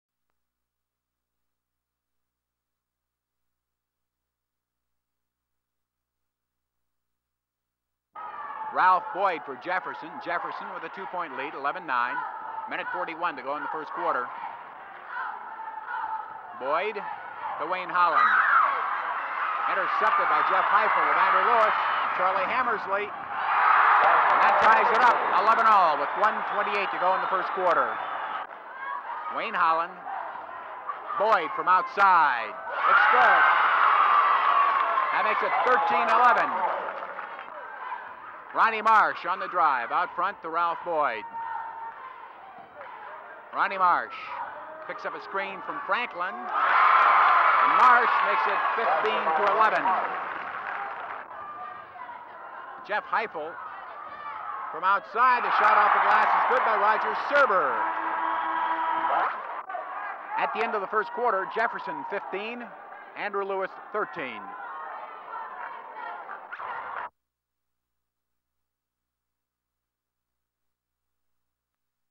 02/24/70 Western High School tournament begins
1672
SPORTS USE: 6:00
2/24/70 BW
LEAD SECTION
LIVE GOOD EVENING, EVERYONE. HERES OUR REPORT ON SPORTS.
 (WESTERN TOURNAMENT BEGINS)
LIVE JEFFERSON...AND, E.C. GLASS---HAVE, REACHED...THE "SEMI-FINALS"---(OF, THE "WESTERN-DISTRICT"...HIGH-SCHOOL, BASKETBALL-TOURNAMENT.)
 (AT, THE SALEM...ROANOKE-VALLEY, CIVIC-CENTER---THIS-AFTERNOON)---GLASS...BEAT, HALIFAX-COUNTY...63-TO-51.
 (AND)---JEFFERSON...DOWNED, ANDREW-LEWIS...51-TO-45---(AS, MIKE FRANKLIN...SCORED, 20-POINTS.)
LIVE CHARLIE HAMMERSLEY...HAD, "21"---(FOR, ANDREW-LEWIS.)
 HERES, A LOOK...AT, SOME---OF, THE ACTION---(DURING, THE "FIRST-QUARTER.") (X)
COLOR-FILM SOUND BASKETBALL (1:05) (OPEN: "RALPH BOYD FOR JEFFERSON. ")
 (CLOSE: "...JEFFERSON 15---ANDREW LEWIS 13.") (X)
LIVE
LIVE THE FINAL-SCORE---(JEFFERSON, 51. ANDREW-LEWIS, 45.)
 ---0---
 2-OTHER..."FIRST-ROUND" GAMES---ARE, SCHEDULED...FOR, TONIGHT.
 WILLIAM-FLEMING...PLAYS, MARTINSVILLE---(AT, 7 OCLOCK.)
 (AND)---PATRICK-HENRY...MEETS, GEORGE-WASHINGTON, OF DANVILLE---(AT, 9.)
LIVE